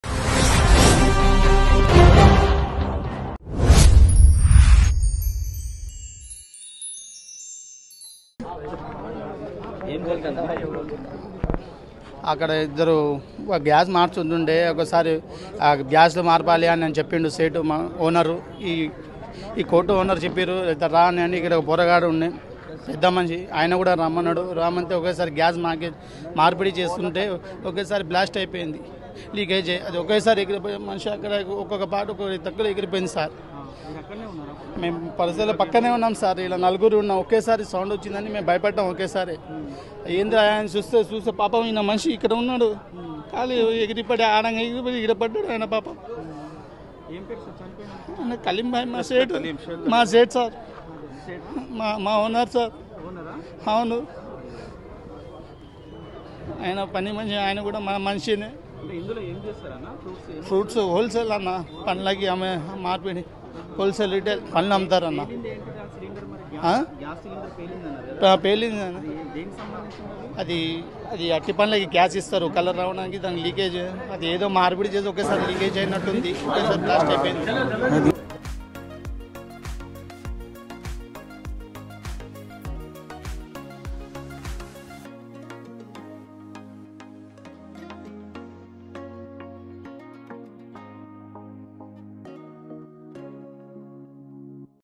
నల్గొండలో దారుణం.. ఏసీ సిలిండర్ పేలి ఇద్దరు మృతి || AC Gas Cylinder Explosion at Nalgonda || RTV
నల్లగొండలో ని బర్కత్ పురా కాలనీ లో దారుణమైన సంఘటన చోటు చేసుకుంది.పండ్ల కోల్డ్ స్టోరేజి లో ఏసి గ్యాస్ సిలిండర్ మారుస్తుండగా పేలి పొయ్యిన్ది. అక్కడ ఉన్న ఇద్దరు వ్యక్తులు ముక్కలు ముక్కలుగా అయిపోయి చనిపోయారు. ఈ సంఘటనలో కోల్డ్ స్టోరేజి ఓనర్ షేక్ కలీమ్,అందులో పనిచేసే వ్యక్తి సాజిద్ మృతులగా గుర్తించారు.